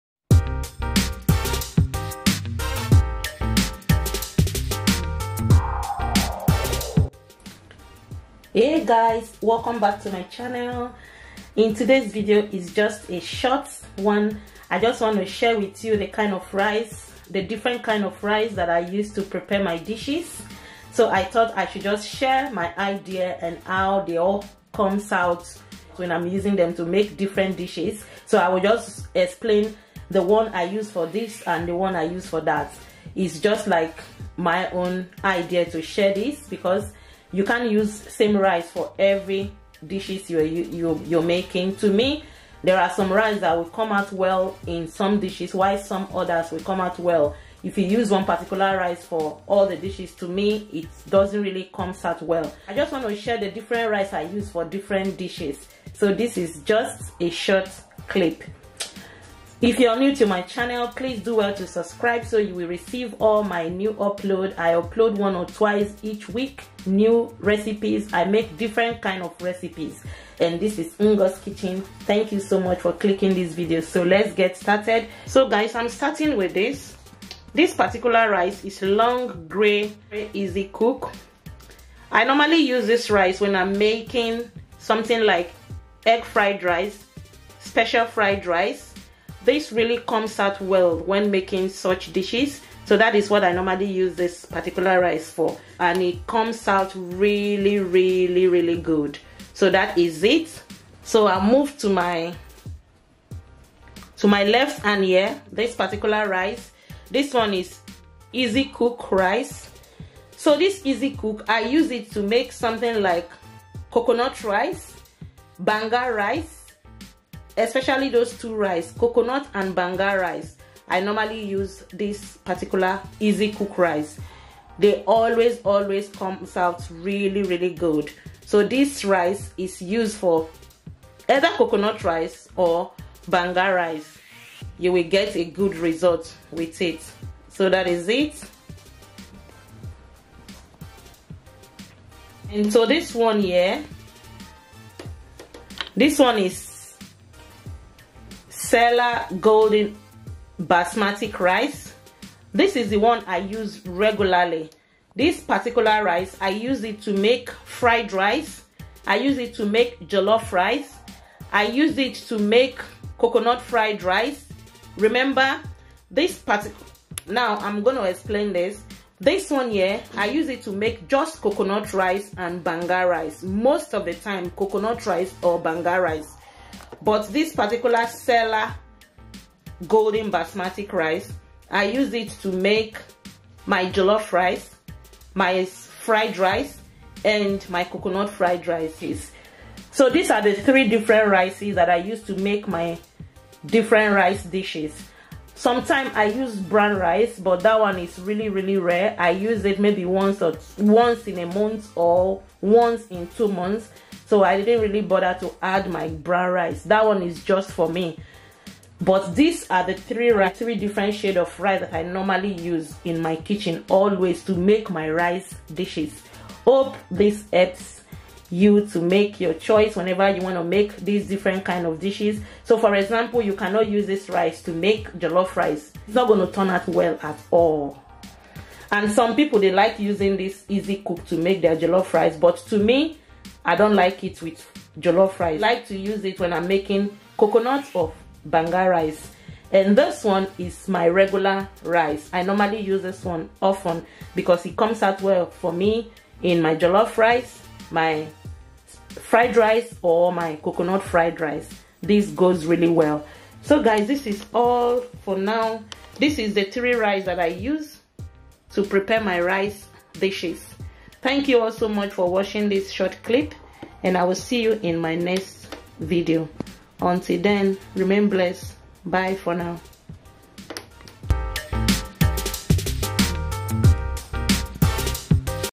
The 3 Different Type Of Rice I Use When Making My Rice Dishes
In today's video I will be showing you the 3 different type of rice that I use in making my rice dishes.

These are the names of the rice
▪︎ Long grain easy cook rice - I normally use this rice in making my special fried rice,  egg fried or any Chinese rice.

▪︎ Easy cook rice - this is the typical easy cook rice, I use this type of rice when making coconut rice or banga rice

▪︎ Golden sella basmati rice - I use this type of rice when making jellof rice,  fried rice or coconut fried rice.

▪︎ Lastly,  I use brown rice but not frequently so I didn't list it.